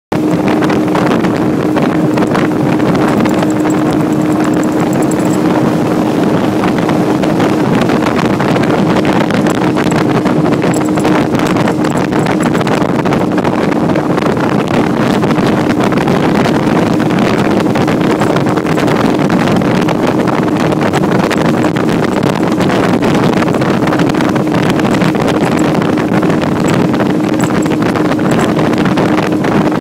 Rylee on the Boat
Rylee my Black and Tan Coonhound likes to ride on the front of the boat with her ears and lips flapping in the wind!  :)